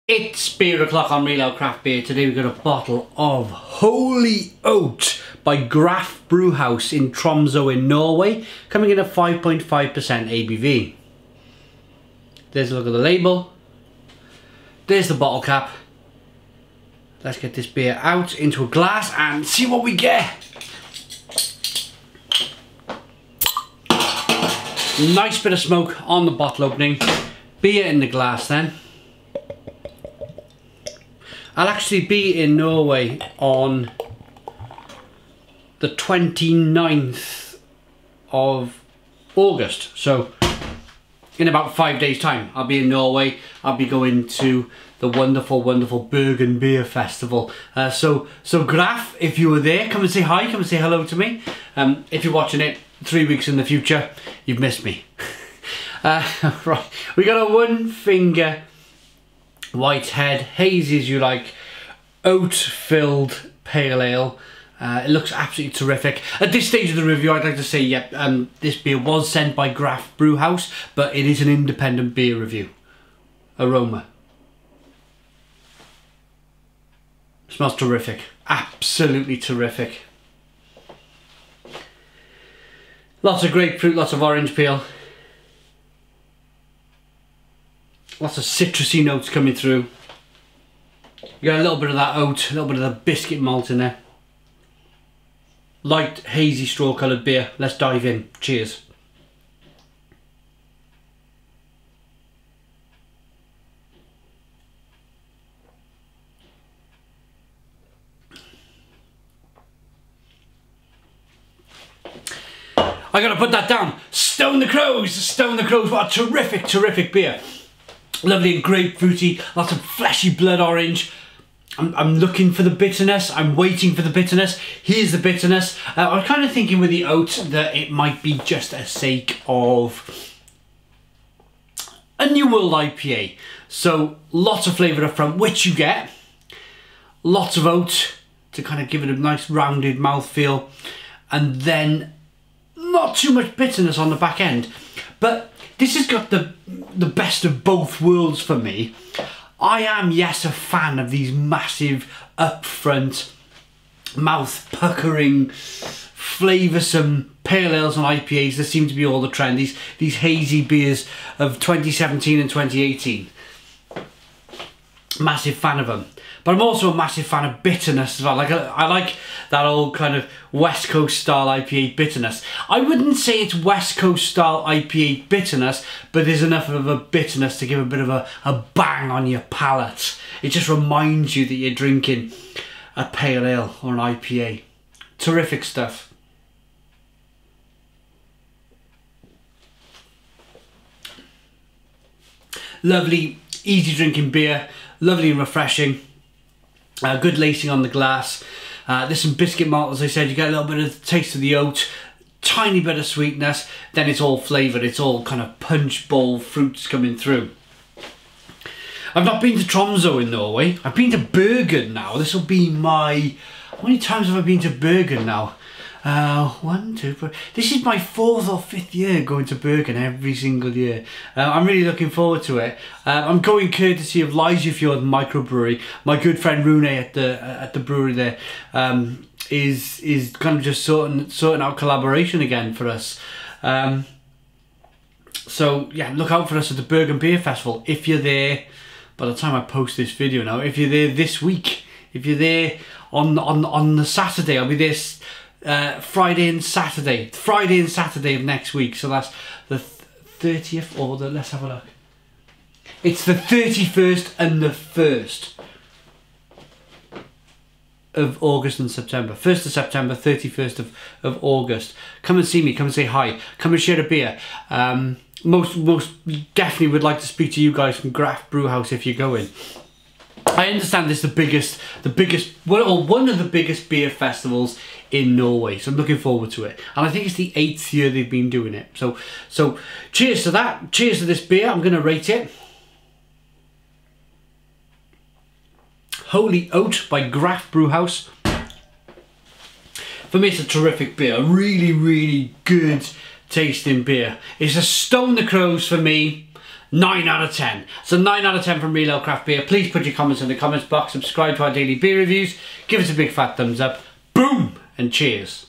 Graff Holy Oat Silkemyk Pale Ale By Graff Brygghus | Norwegian Craft Beer Review
Recorded In 4K Ultra HD Real Ale Craft Beer Reviews Graff Holy Oat Silkemyk Pale Ale By Graff Brygghus | Norwegian Craft Beer Review